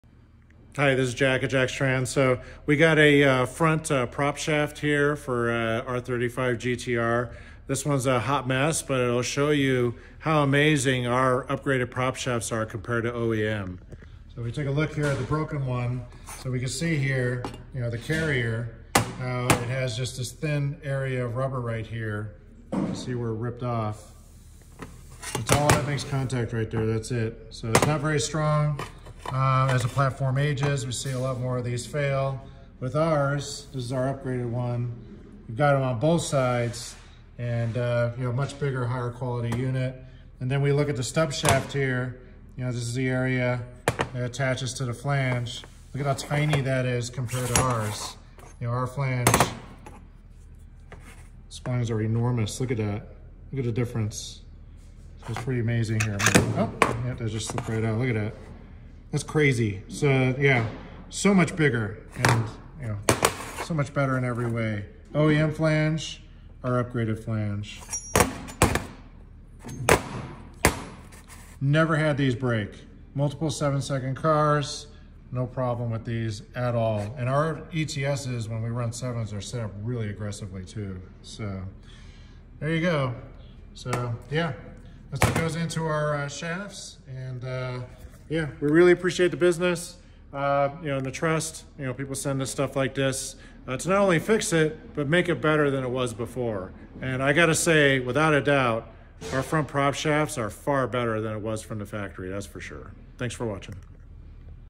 Front Prop Shaft, Drive Shaft Repair / Upgrade, R35 Nissan GT-R, Made In-House- Jacks Transmissions
Comparing weak and aging OEM Nissan prop shaft with our prop shaft driveshaft.  This is the shaft that drives the front diff.